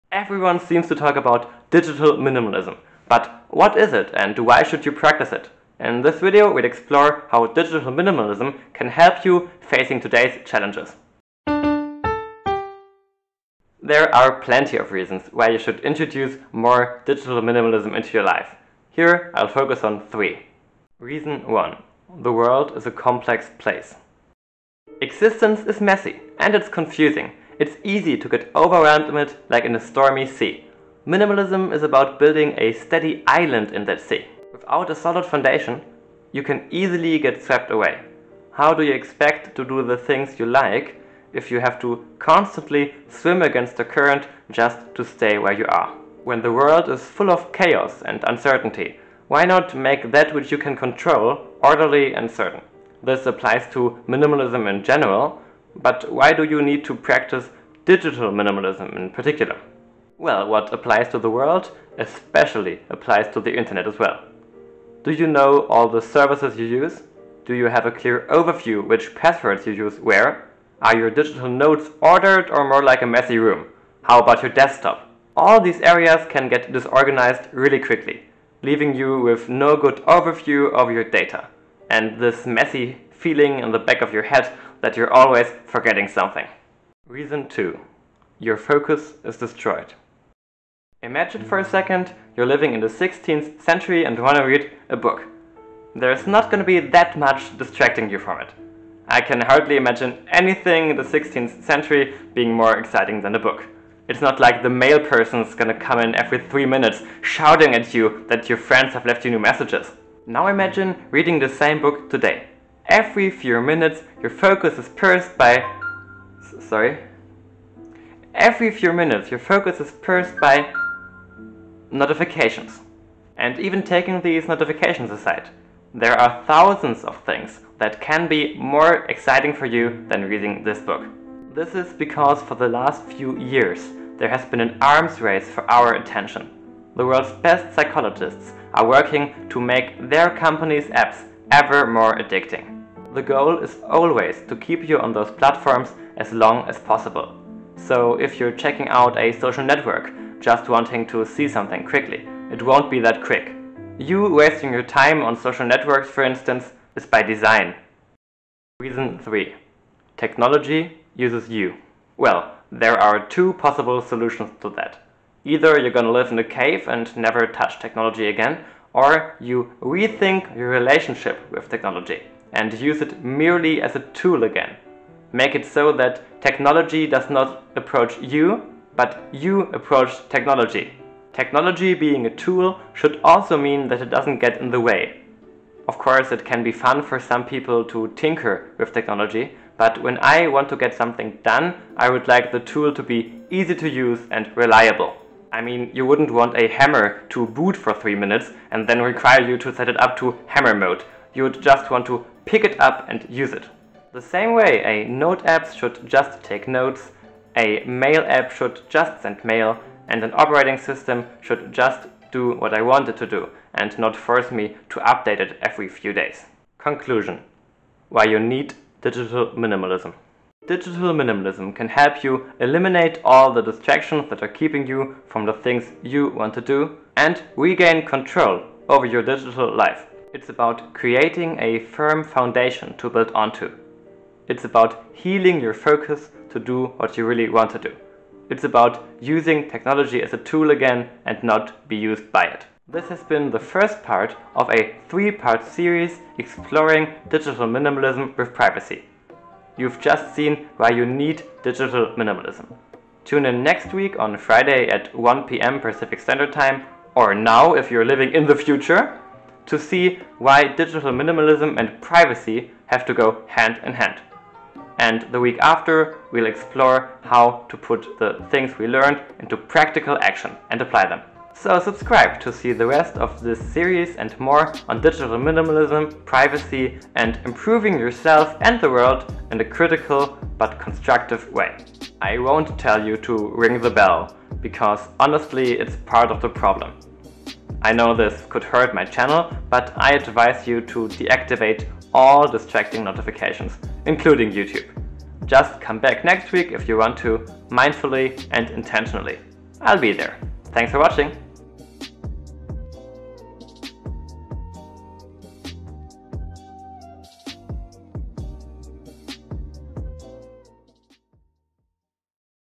3 Reasons for Digital Minimalism.
Everyone is talking about Digital Minimalism. But what is this all about? I give three reasons why introducing more Digital Minimalism into your life can help you facing today's challenges.

0:00 Intro
0:20 Reason I: The world is a complex place
1:26 Reason II: Your focus is destroyed
2:38 Reason III: Technology uses you
3:36 Conclusion: Why you need Digital Minimalism
4:02 Outro

 ✅  ICON SOURCES ✅

🎵   MUSIC SOURCES 🎵